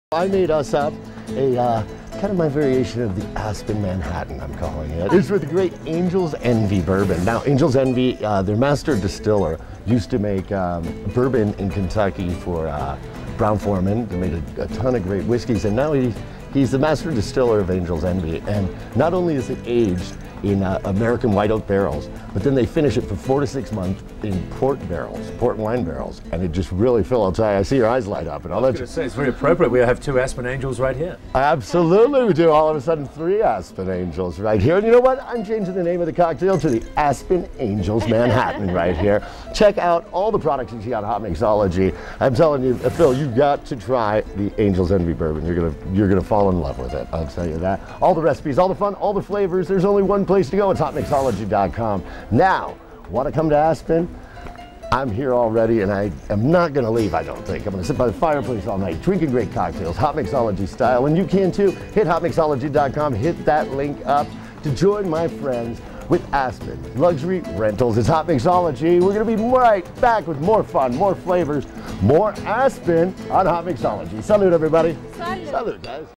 ANGELS ENVY in ASPEN
Angels Envy Bourbon makes this cocktail just perfect in Aspen Colorado on Hotmixology